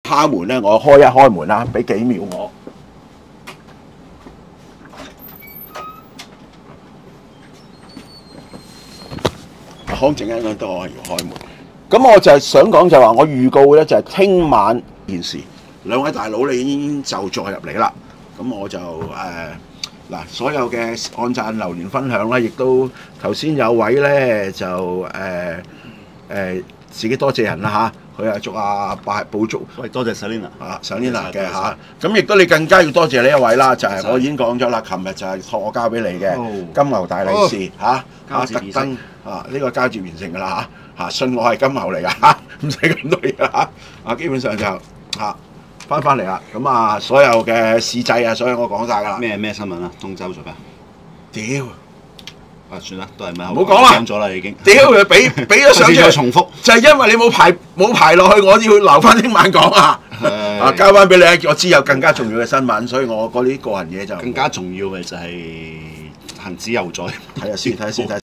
20241009 圍爐賤種陳榮泰卑賤到叫打女人偷錢賊林匡正做大佬，偷錢賊出咗糧有錢剪髮都係幫襯啲街邊阿伯廿蚊剪一剪，個柒頭剪到又長又短，一睇就知唔撚識剪碎腳求撚其剪。乜唔係好多錢買波衫咩？入髮型屋都負
擔唔起
QB house 都算連鎖店，都負擔唔起
連續 3晚著陳年舊衫，啲波衫呢？買咗幾件新波衫架，還咗囉